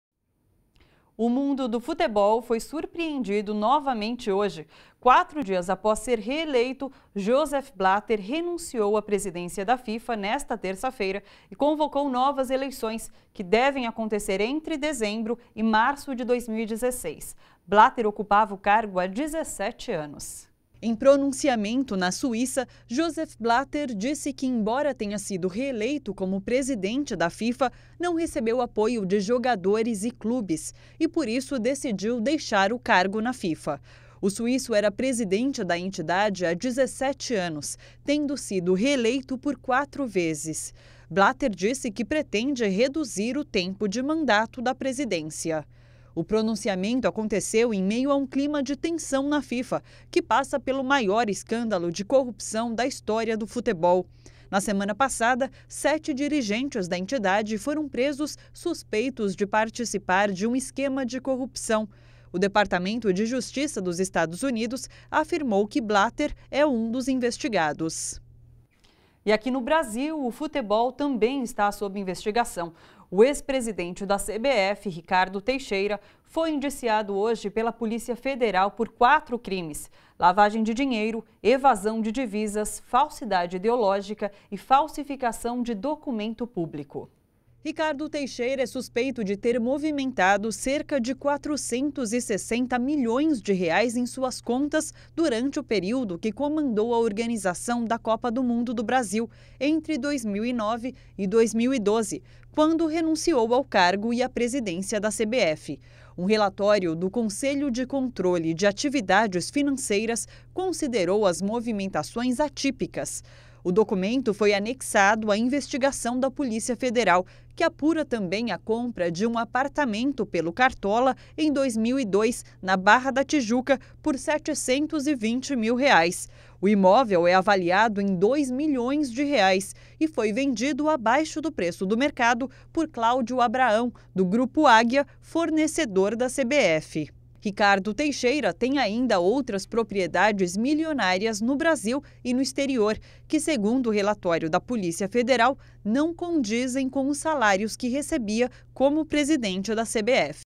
Ex-presidente da CBF é indiciado pela Polícia Federal
Ricardo Teixeira é acusado de lavagem de dinheiro, evasão de dívidas, falsidade ideológica e falsificação de documento público.